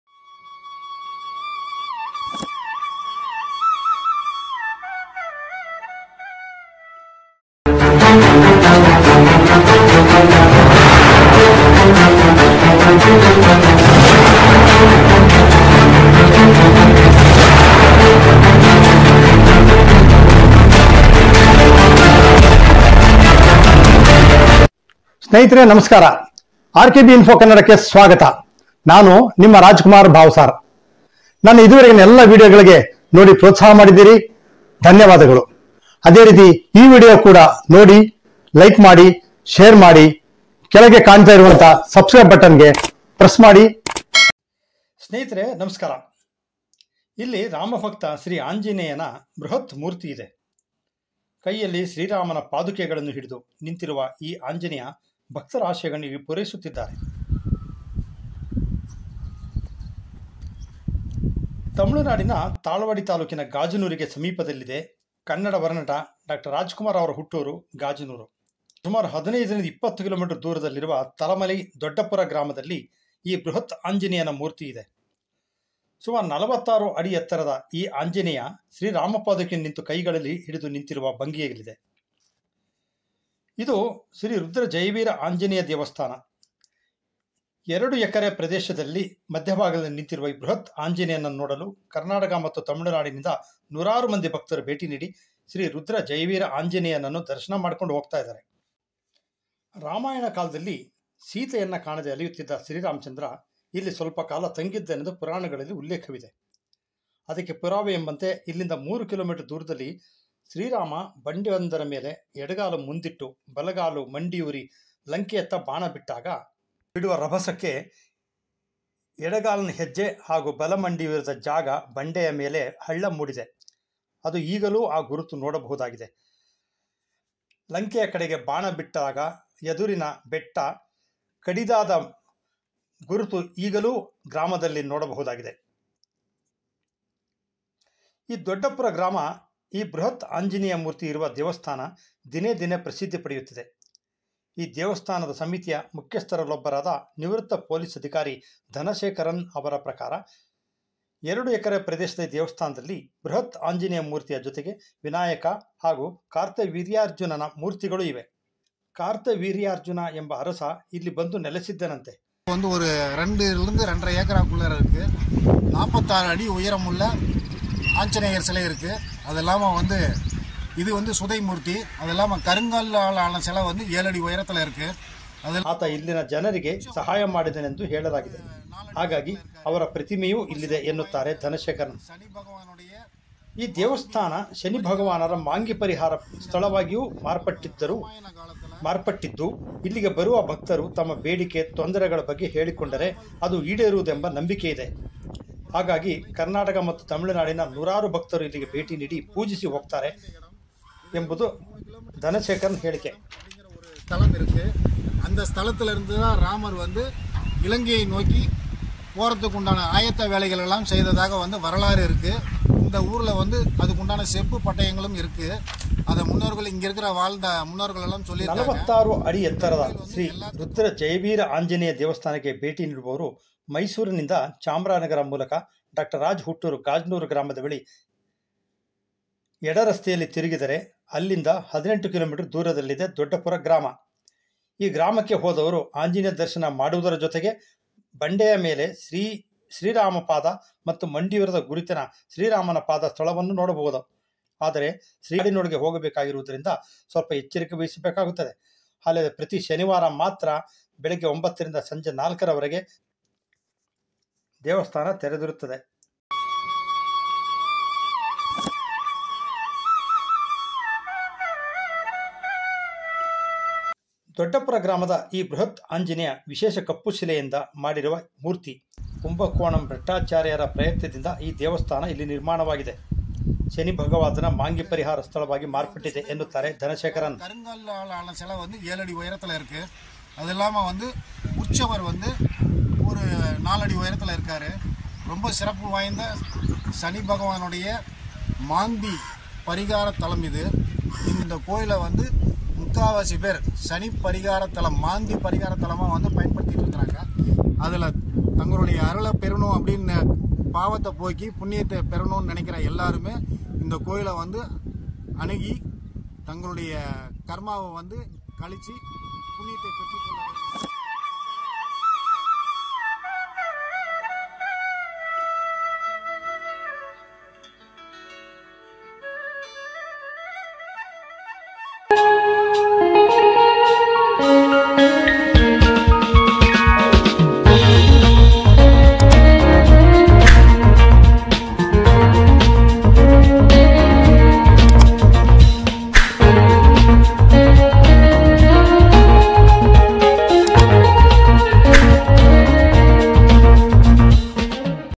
ಗಮನ ಸೆಳೆಯುತ್ತಿರುವ ಬೃಹತ್ ಆಂಜನೇಯ ಮೂರ್ತಿ ಎತ್ತರ 46 ಅಡಿ ಡಾ.ರಾಜ್ ಹುಟ್ಟೂರು ಗಾಜನೂರು ಬಳಿ I RKB INFO KANNADA
ಗಮನ ಸೆಳೆಯುತ್ತಿರುವ ಬೃಹತ್ ಆಂಜನೇಯ ಮೂರ್ತಿ ಎತ್ತರ 46 ಅಡಿ ಡಾ.ರಾಜ್ ಹುಟ್ಟೂರು ಗಾಜನೂರು ಬಳಿ I 46 feet tall huge Sri Anjaneya Idol Near Gajanur of Dr. Rajkumar's Hometown in Tamilnadu I RKB INFO KANNADA I 2024